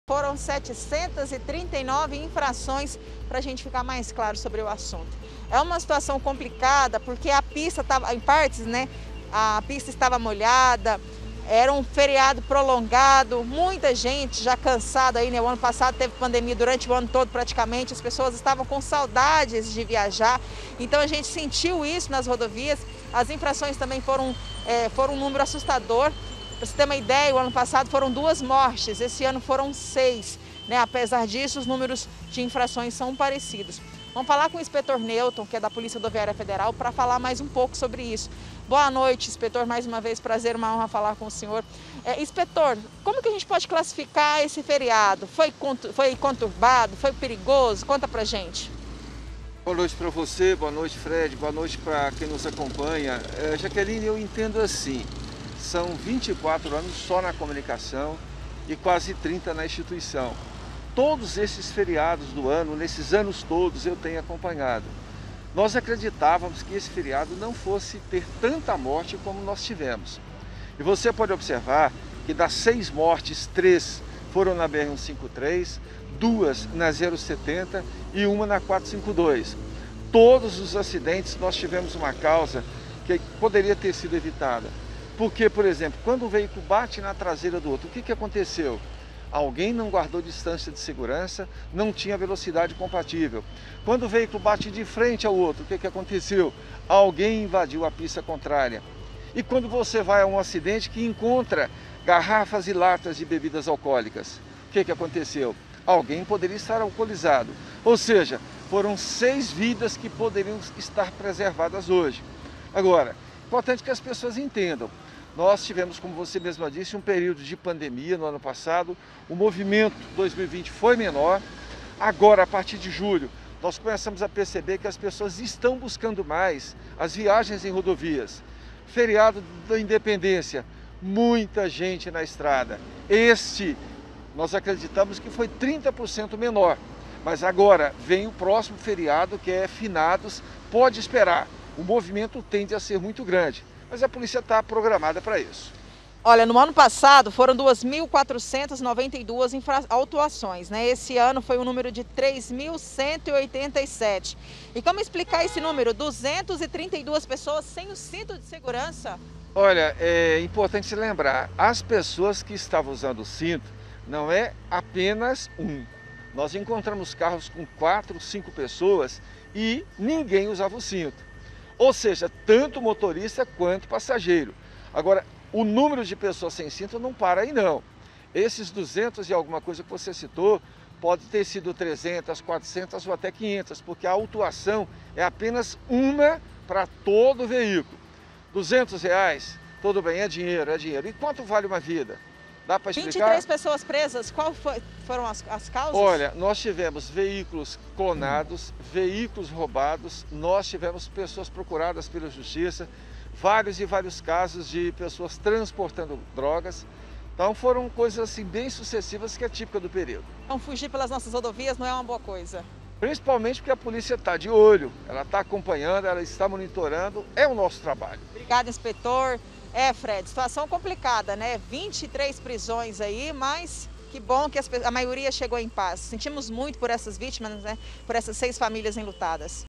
FERIADO PROLONGADO: PRF REGISTRA 739 ULTRAPASSAGENS PROIBIDAS NAS BR'S
POLÍCIA RODOVIÁRIA FEDERAL REGISTRA MAIS DE SETECENTAS ULTRAPASSAGENS PROIBIDAS NAS BR'S EM GOIÁS DURANTE O FERIADO PROLONGADO.